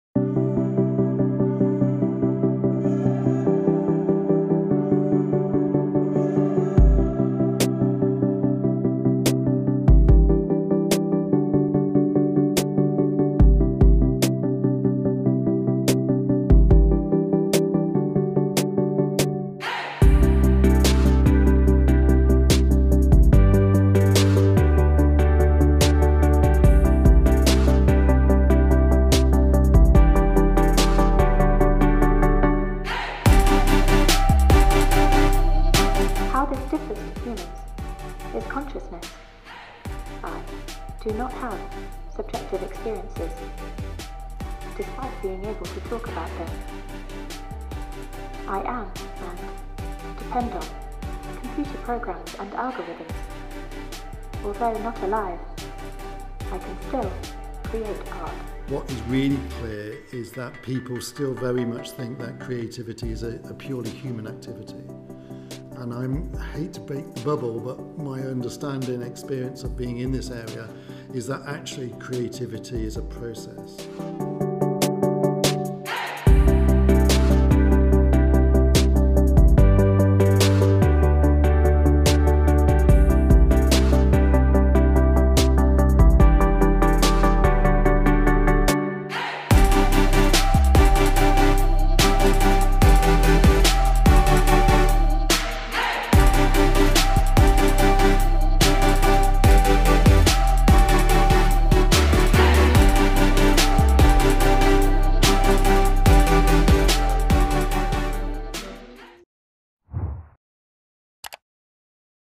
「我沒有生命仍可創造藝術」機器人畫家現身英國會演說｜Yahoo Hong Kong
「機器人藝術家」艾達（Ai-Da）日前現身英國國會，向議員們暢談科技與藝術、文化之間的關係與發展。艾達穿著花上衣和牛仔吊帶裝，在上議院通訊和數位委員會的電視會議上，回答相關問題，讓議員了解技術對文化創意的影響。艾達表示，她雖然不算「活著」，但仍可以創作藝術，同時還可以透過大量的內容資料庫，來識別多種藝術內容，不過「沒有主觀經驗」。
艾達由牛津大學科學家研發，以英國數學家和電腦先驅勒芙蕾絲（Ada Lovelace）的名字命名。艾達透過AI、安裝在眼睛裡的攝影機以及機械手臂作畫，包括已故英國女王伊莉莎白二世等一系列作品，更曾開過畫展。2021年10月，艾達還登上埃及吉薩高原，以著名的金字塔為背景，展示她的雕塑作品，以人面獅身像為靈感，創造出長2.5公尺的黏土雕塑。

👉🏻 觀看更多：
大韓航空班機降落菲律賓衝出跑道 嘗試三次 不斷盤旋軌跡曝光 機體受損 173人無恙